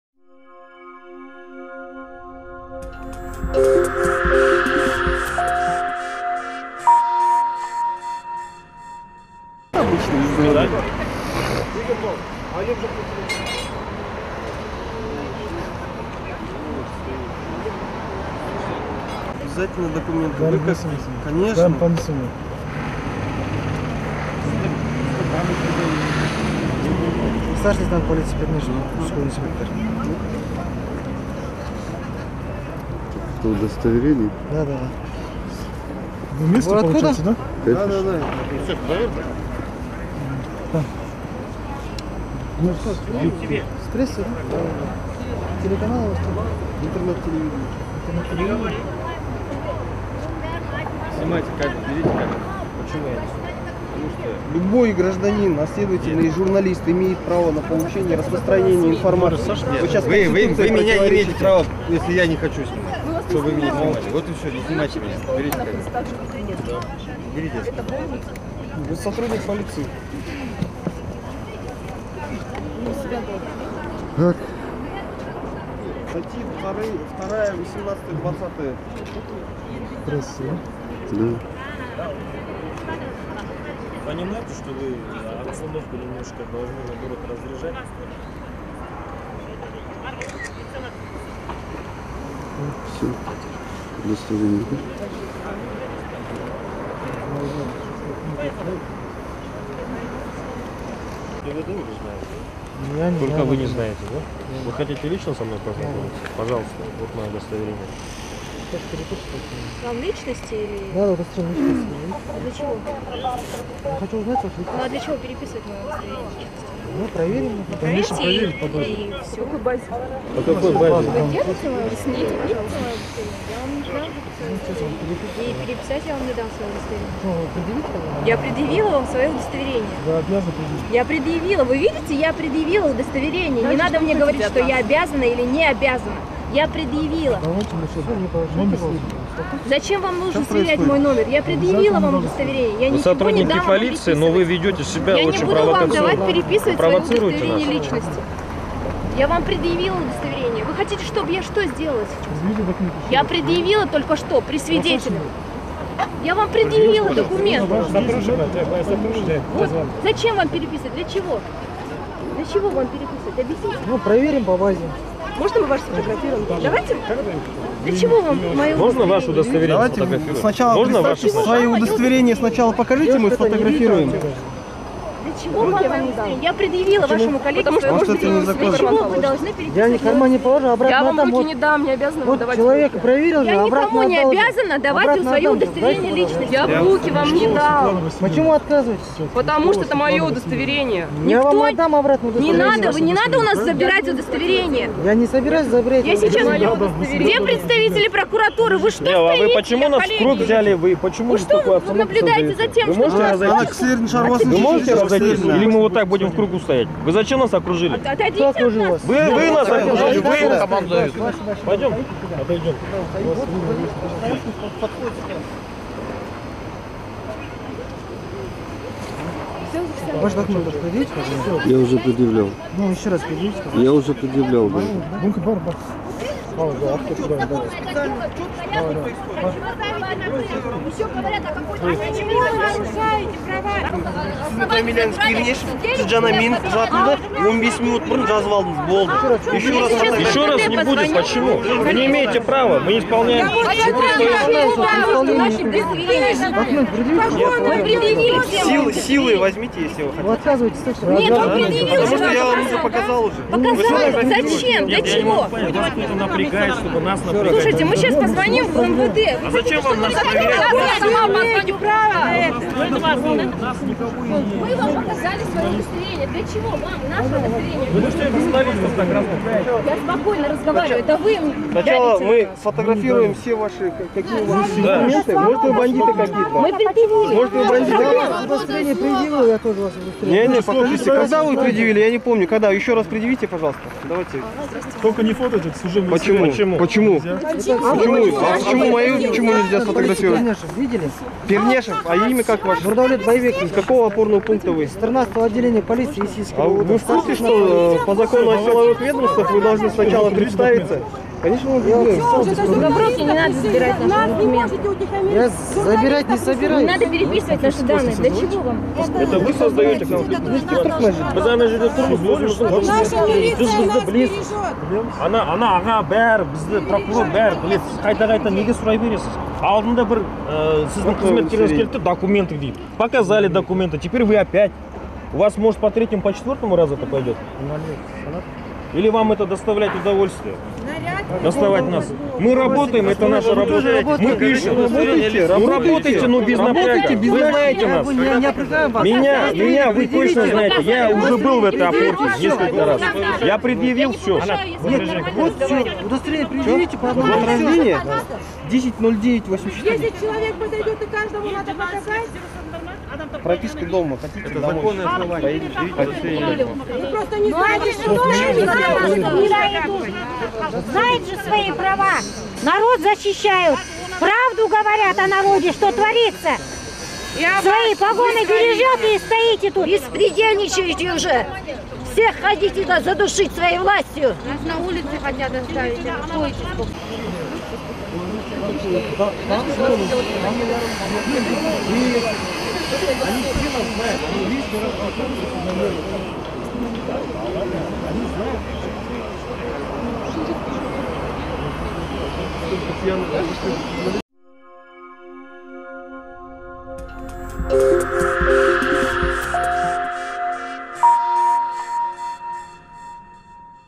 Полиция Астаны против журналистов / 1612
На этих кадрах те самые полицейские из Есильского управления полиции Астаны, которые напали на журналистов видеопортала 16/12.